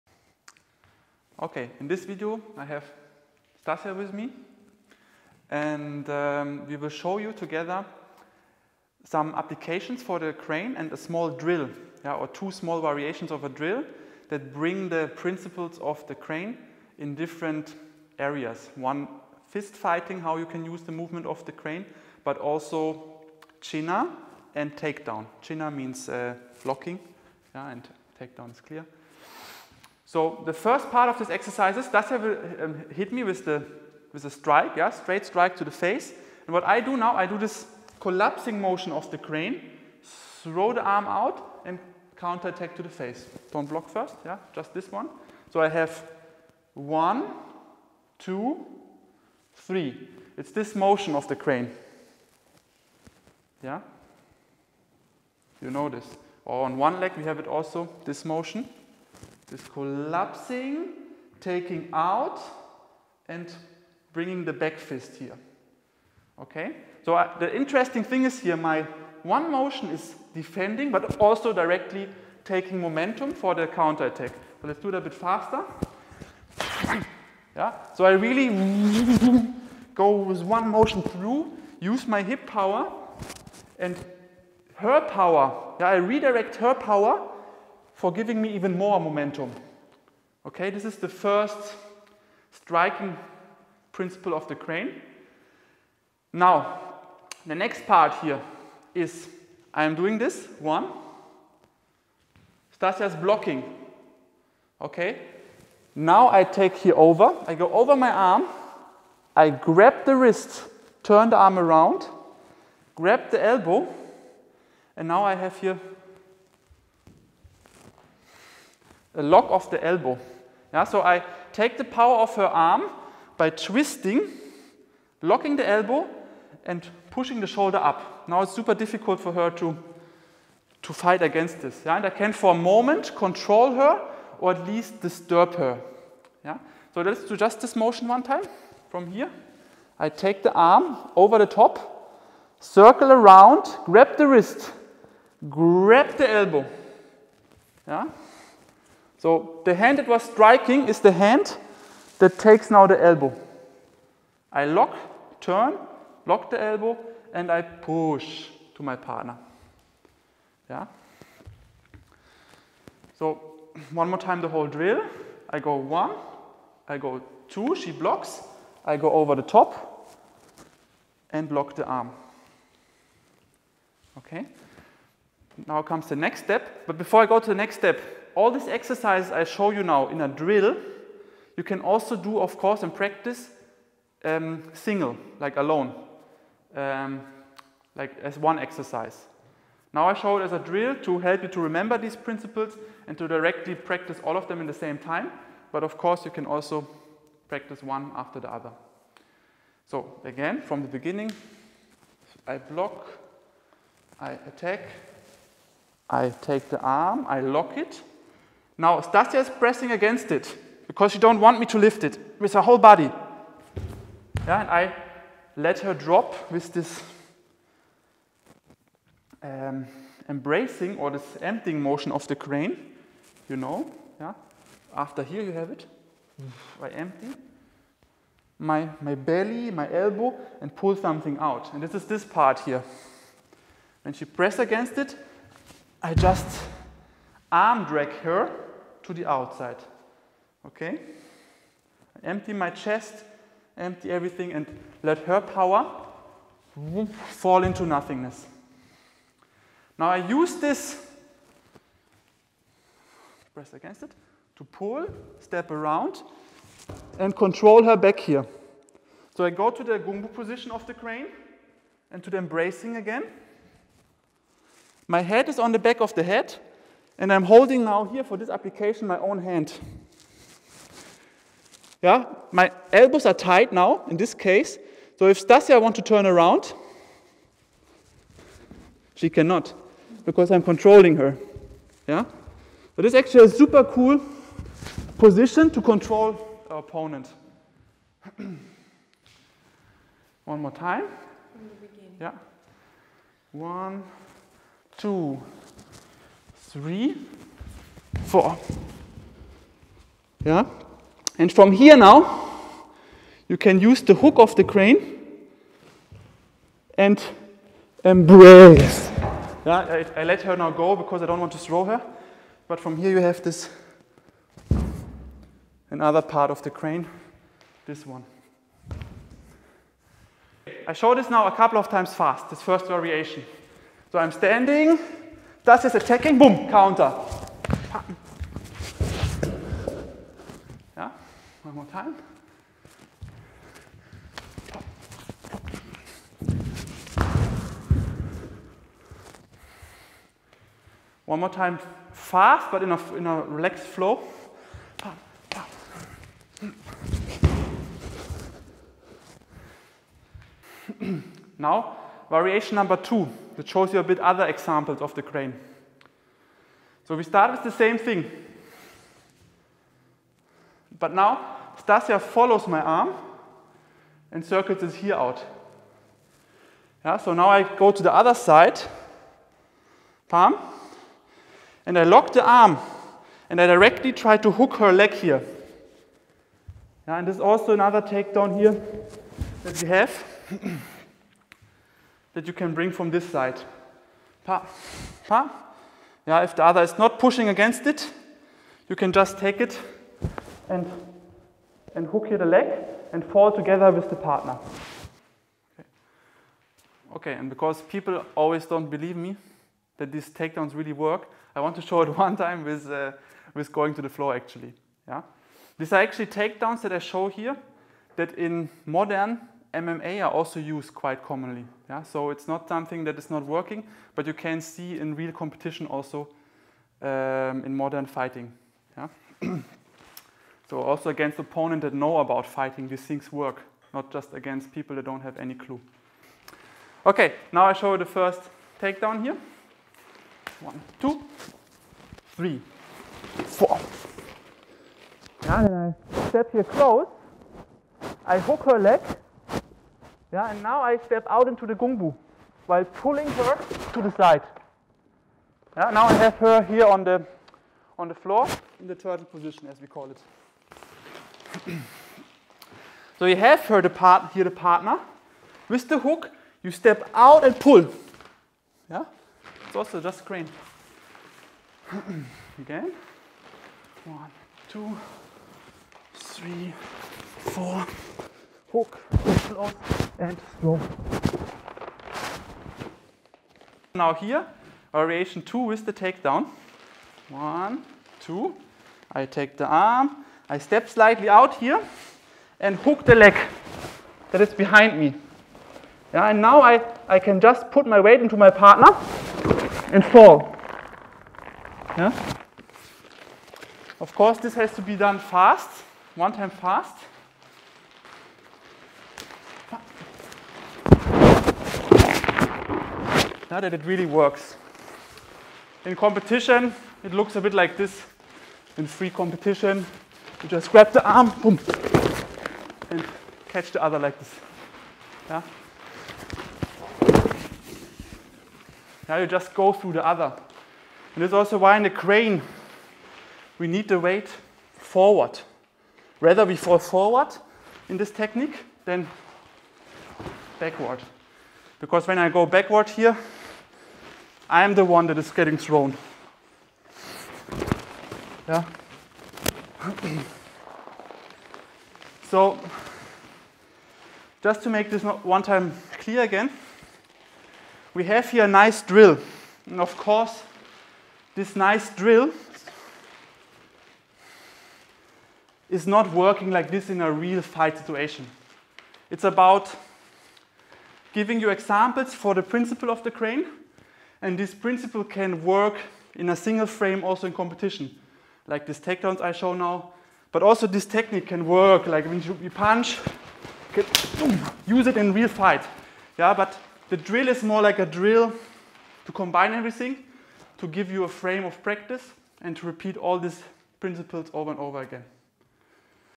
Crane Qi Gong - Applications
Get here some inspiration for the usage of the Crane Principles in Fighting. When the 5 Animals Qi Gong in the first place looks like a movement and health school, you can also find efficient principles useful for a street fight or even modern competition.